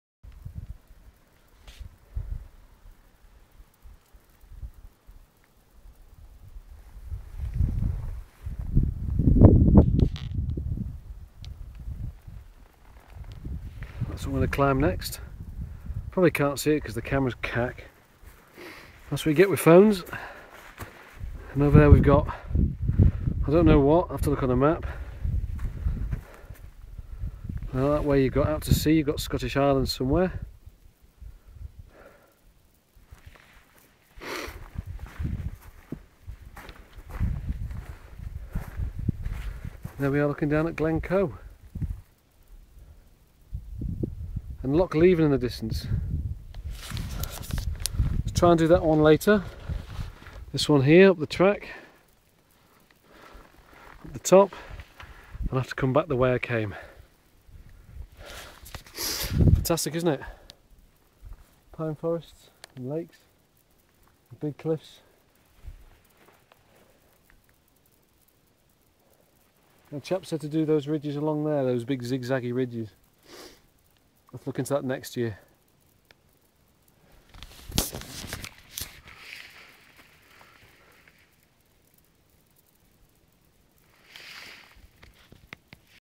Glen Coe 1
a view from the trip up to stob choire nam beith 1102 m. check out the vews, fantastic